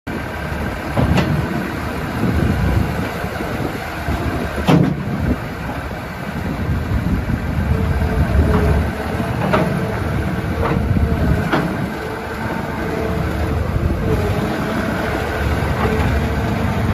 DEERE 260 For Sale
OROPS, Foot Controls, 1 Set of Auxiliary  hydraulics, Manual Front Quick Attach, Hrs. Can't  Read  SN: KV0260A261854

 [JOHN DEERE 260 SKID STEER]